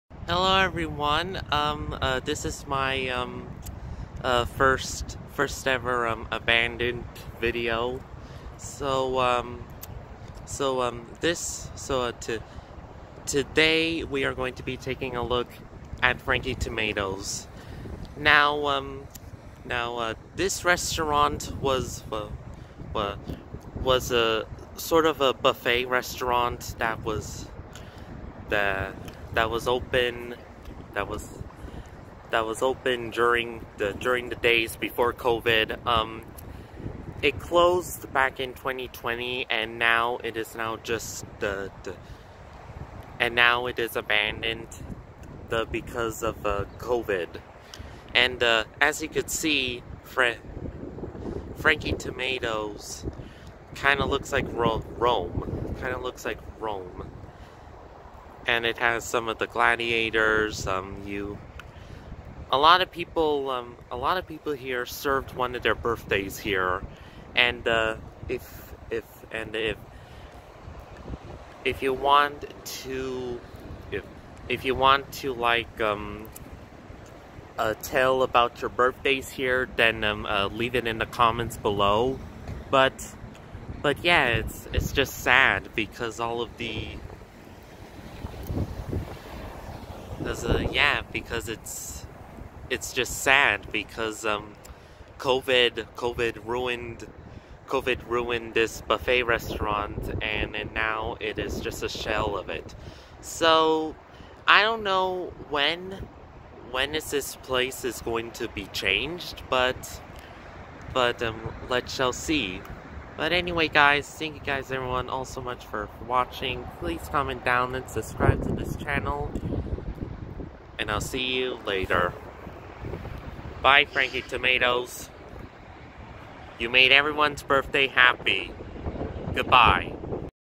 Abandoned Franky Tomato
Franky Tomato was a  Buffet restaurant that have pretty good food  and birthday party thar  it was popular for the  Balloon Tomato in 2020 it was close do to COVID-19 and the reason

If you have ever went to franky Tomato then please leave it in the comments